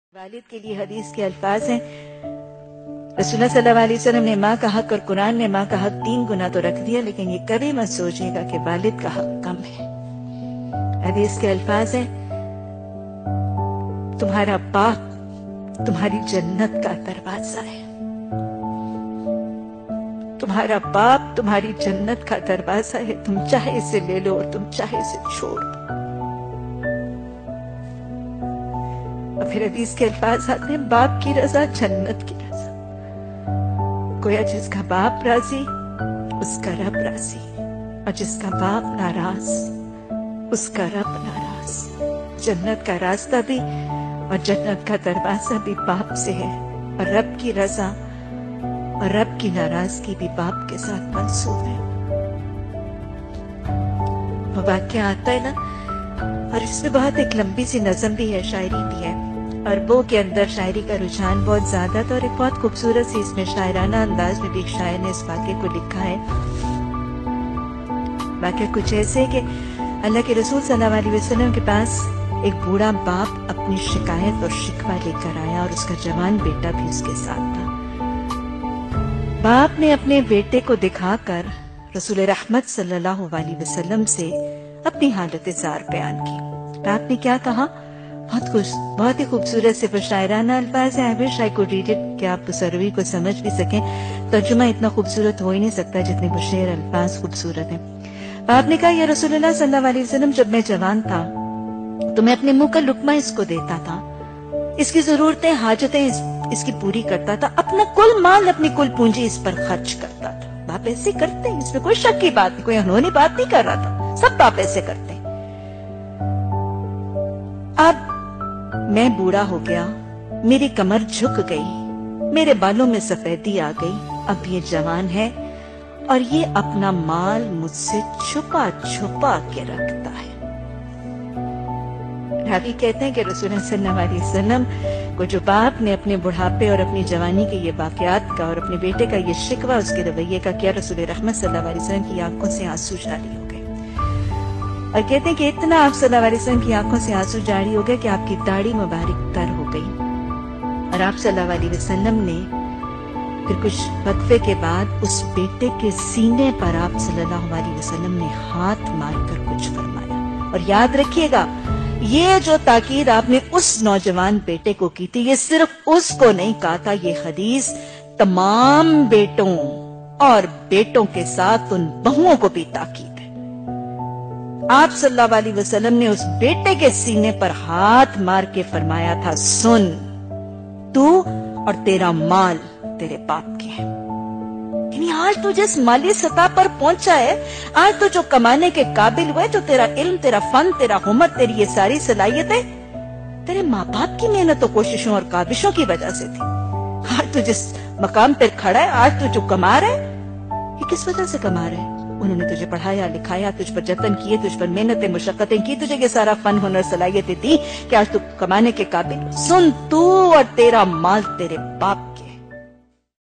BAAP JANNAT KA DARWAZA
Maa ki muhabbat to dunya janti ha par baap ki mohabbat or ehmiat khud baap ban'ne k bad pata lagti ha